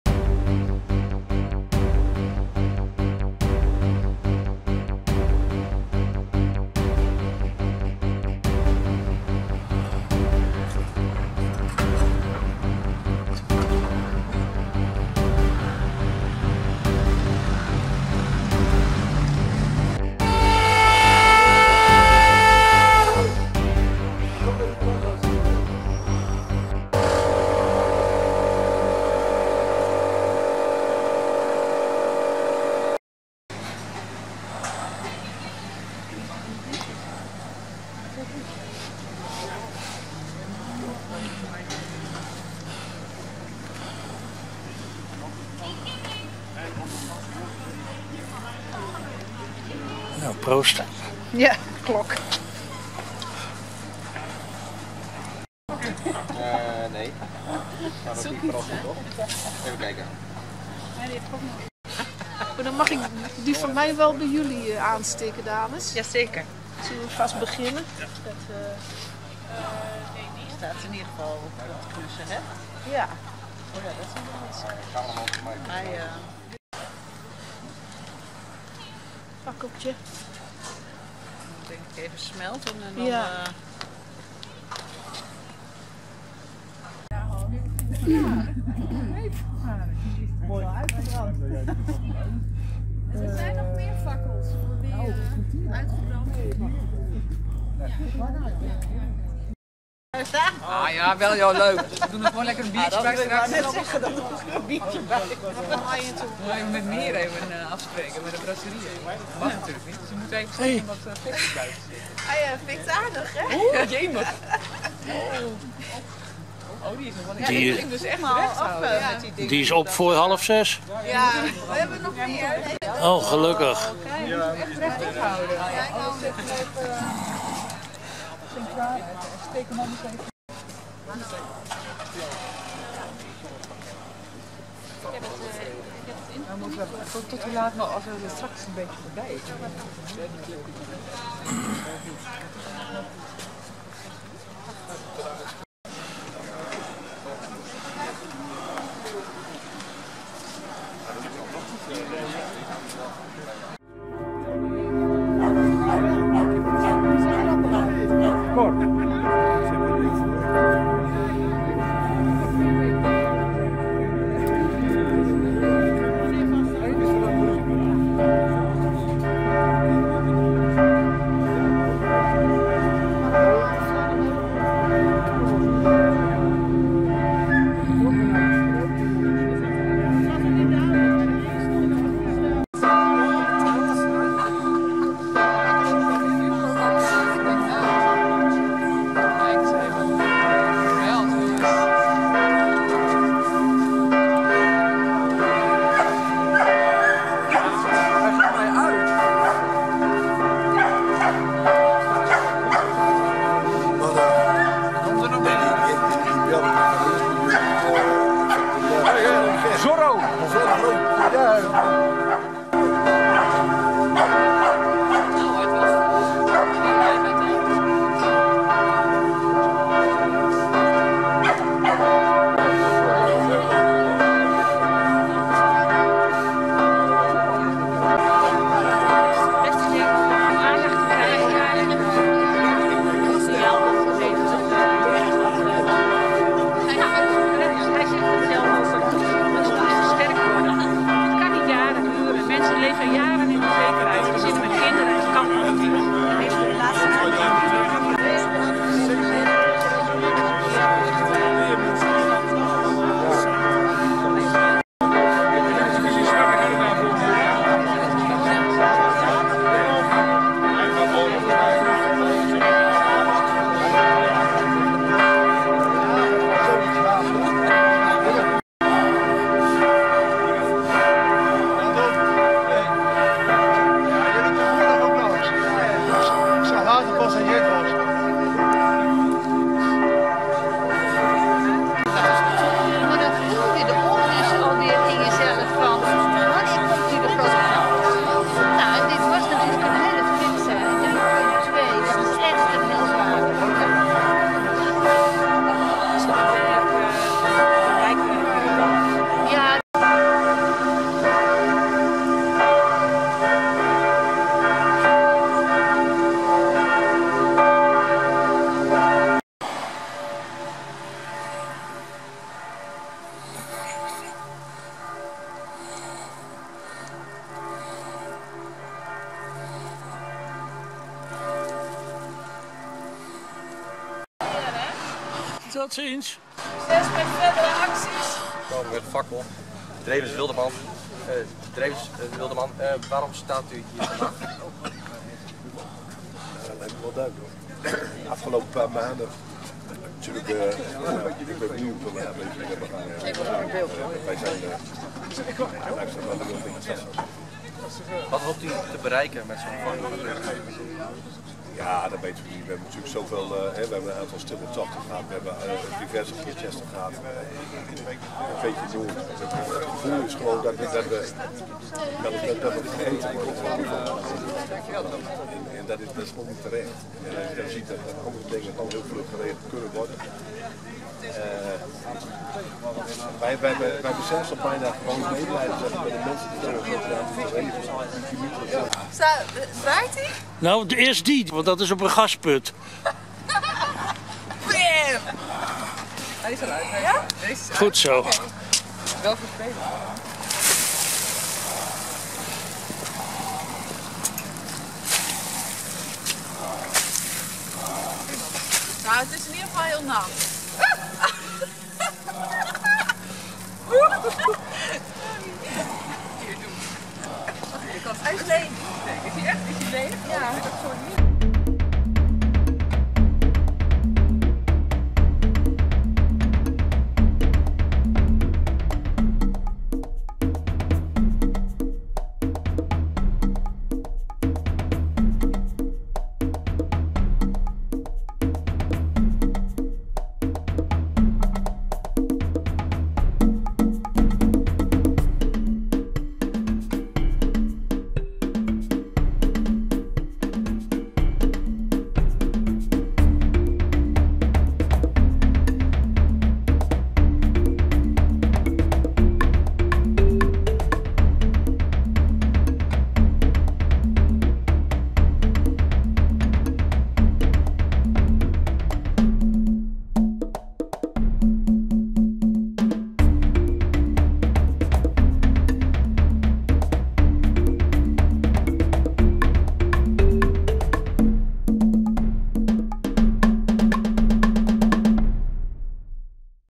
Fakkelprotest Loppersum 17 november 2021
Na de zoveelste aardbeving veroorzaakt door gaswinning (deze keer in Garrelsweer, met een kracht van 3,2 op de schaal van Richter) op 16 november 2021 werden de volgende dag her en der in het hele Groningse bevingsgebied door gedupeerden en sympathisanten als vlammend protest fakkels en kaarsen gebrand, zoals hier in  Loppersum.